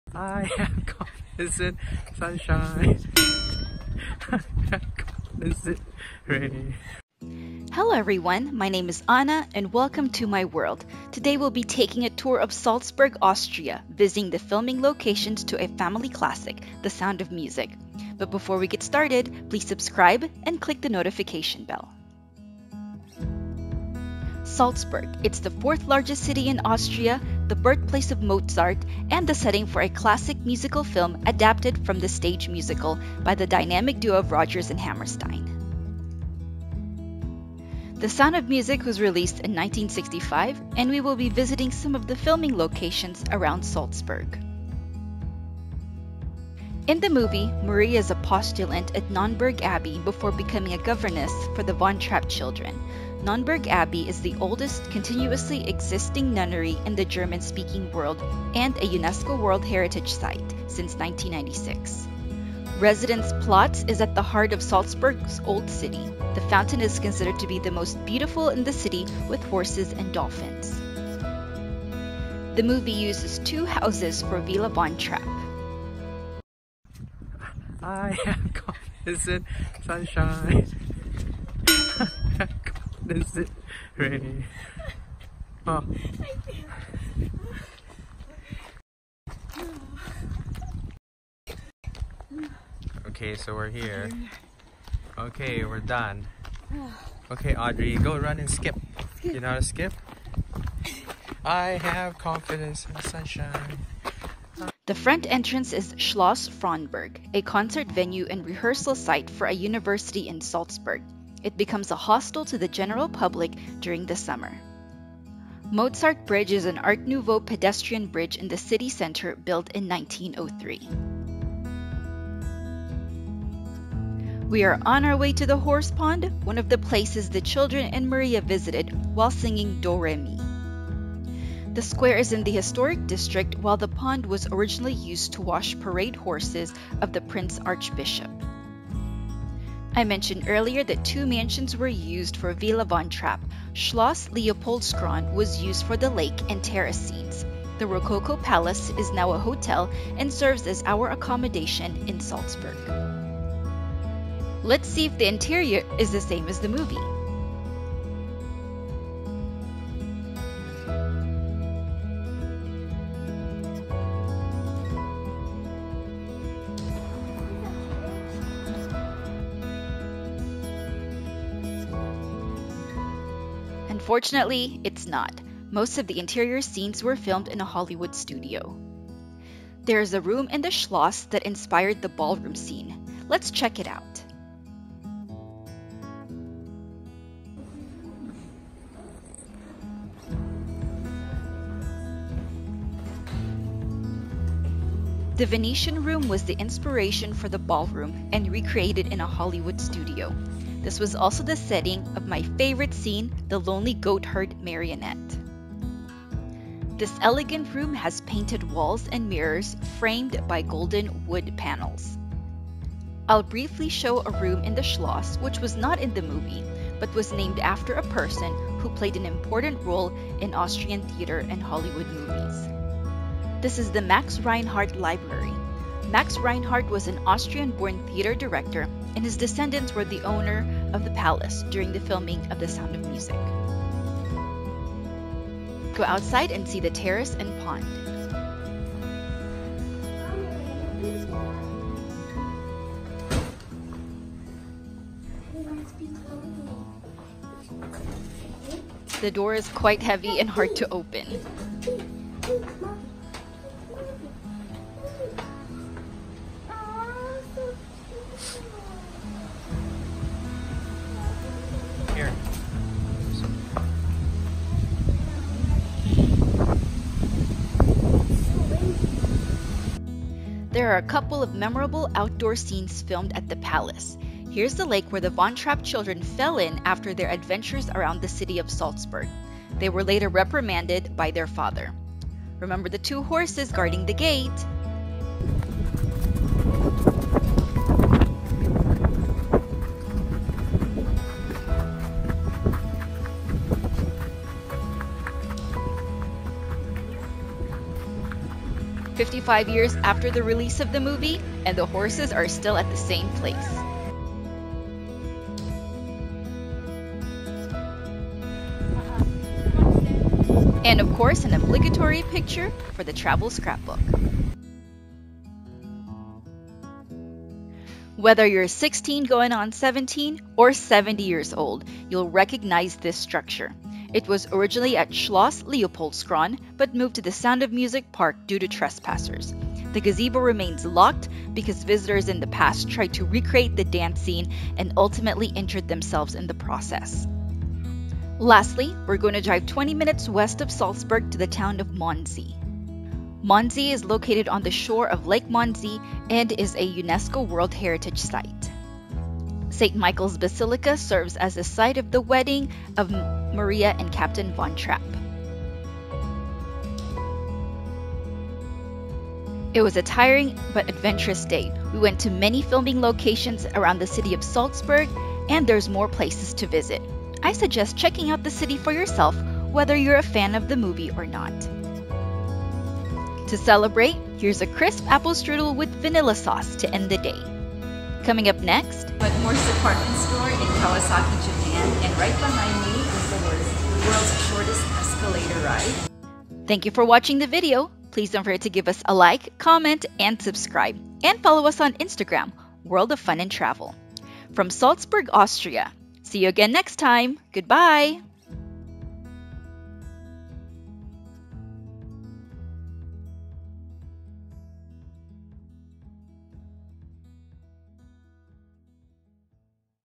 Sound Of Music Filming Locations | Salzburg, Austria | Schloss Leopoldskron
In this episode, we stay at the Von Trapp Family’s home in the movie and visit some of the locations where the movie was filmed as we toured one of the most picturesque cities in Austria.